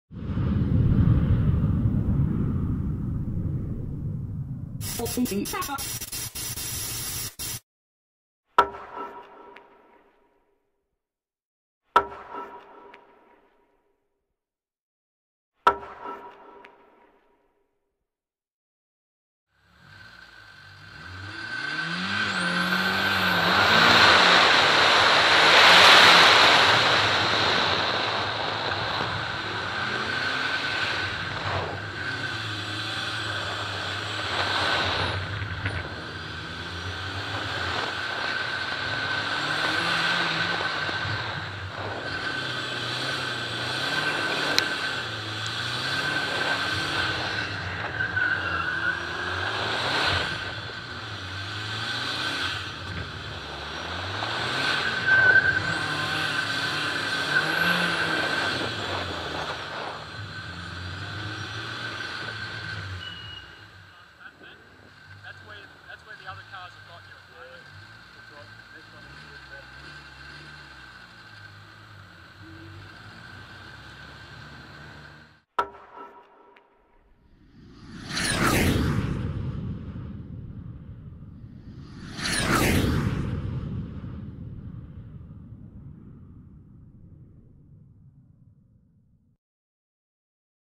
NSW WRX Club - Eastern Creek skid pan John Malkovich EVO 6
NSW WRX Club motorkhana skid pan day. Mitsubishi Lancer Evolution 6. For the full report on this head over